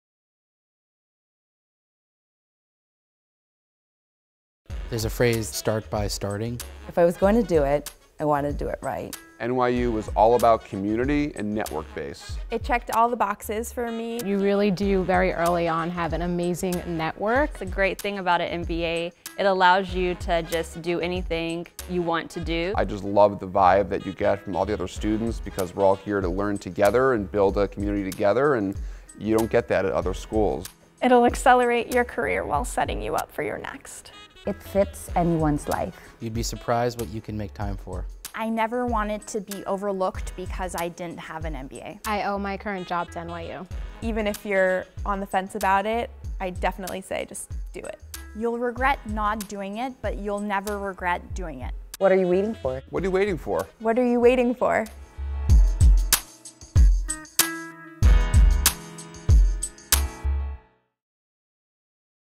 [Part-time MBA] What Are You Waiting For?
Students and alumni talk about the Langone Part-time MBA program at NYU Stern.